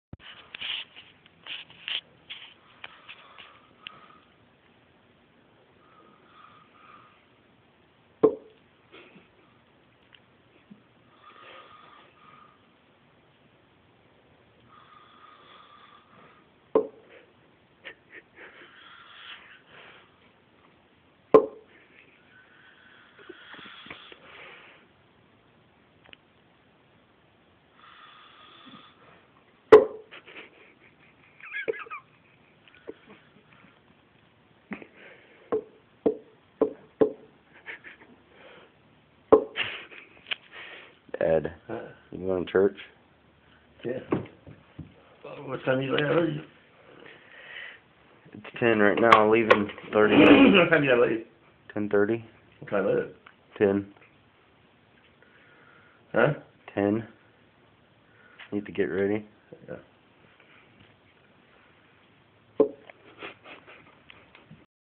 PARENTAL PRANK STARS *DAD* #1
My dad was asleep on the chair and I saw an empty shampoo bottle in the bathroom... I had to!

I gave him a couple knocks, still he didn't wake. I then asked if he was going to church and listen to the speed of his words when he talks...

and then I finish it off with one last knock.